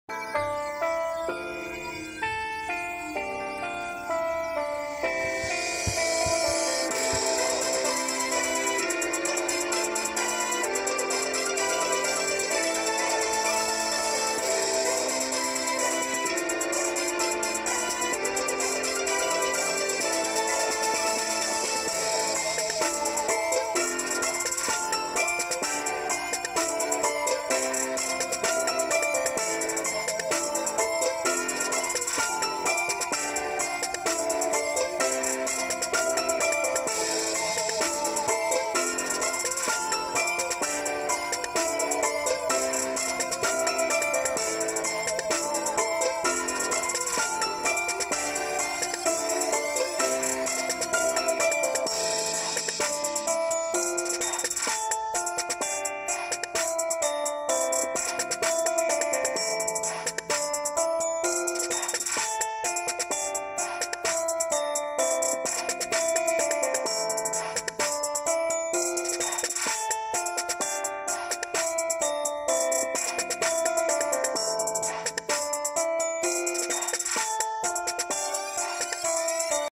Some of my New Year presents unboxing | Frozen 2 surprises unboxing
Hello everyone ❄️
And today I will show you the Frozen 2 surprises unboxing ❄️
What was inside of this packets you can know in this video ❄️
And the most important for you is click to the bell 🔔 to do not forget to watch my new video❄️